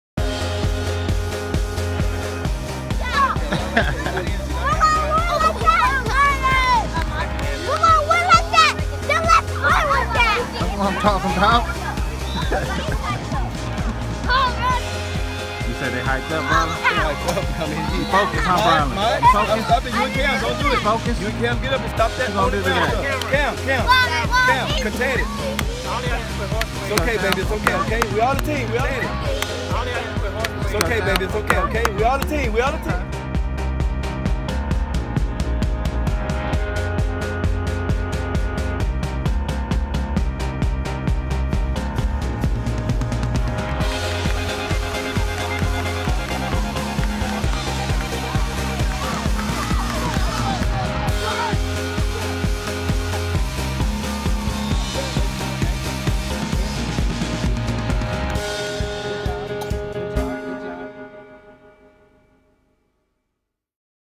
Brylin Stroughter flag football
3 touchdowns good job son